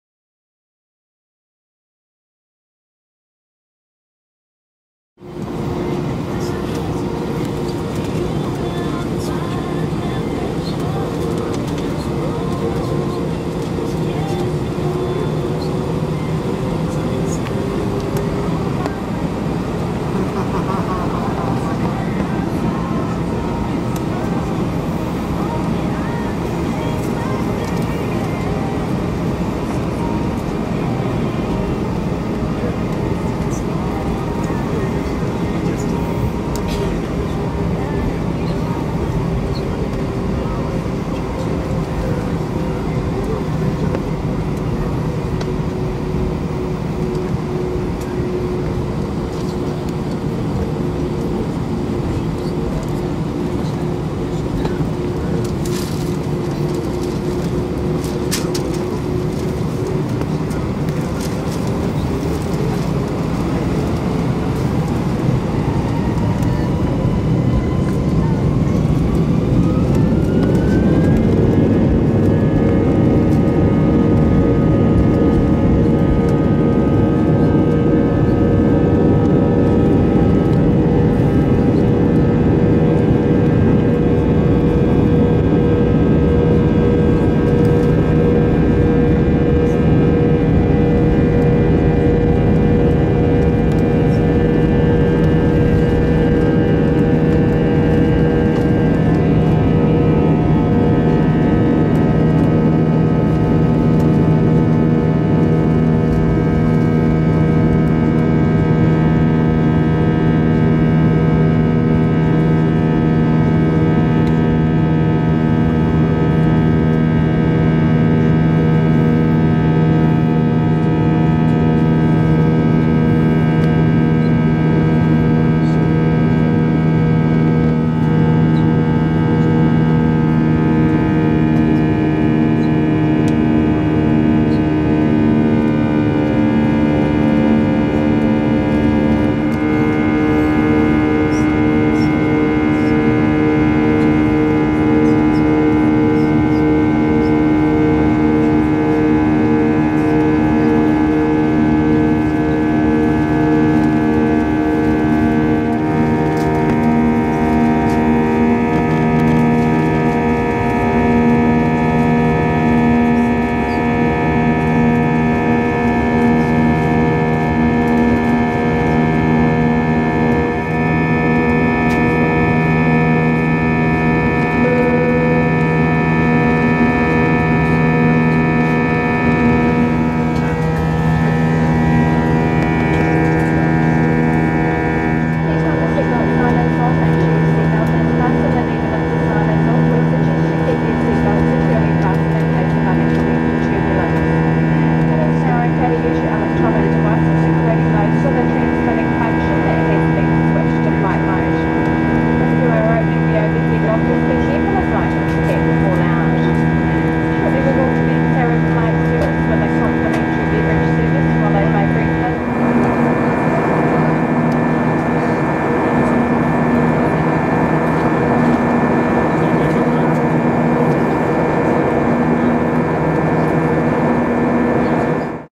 Air New Zealand NZ890 Take Off Mel Chc 31OCT11 short version A320 Leading Edge Engine View
Short version as above from Seat 4a on board code share flight NZ 890 (with Pacific Blue) Melbourne Au to Christchurch New Zealand.  Approx a 3 hour flight and meals provided.

IAE V2500 are beast sounding engines   "OriginalDaVe"